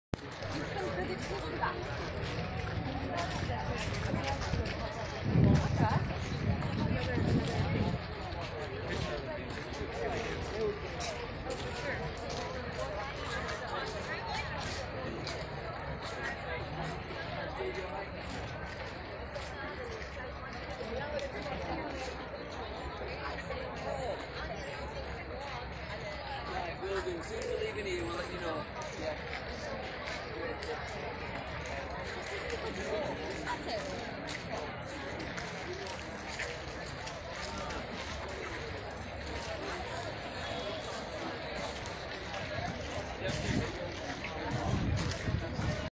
Rehabilitation de Notre-Dame de Paris, Incendie du 15 avril 2019
Le violent incendie du 15 avril 2019 détruit la flèche et la totalité de la toiture couvrant la nef, le chœur et le transept. Il s'agit du plus important sinistre subi par la cathédrale depuis sa construction. Dès lors, Notre-Dame a été fermée au public. La reconstruction à l'identique des parties détruites ou gravement endommagées est décidée en 2020. La réouverture de Notre-Dame au public est prévue le 8 décembre 2024.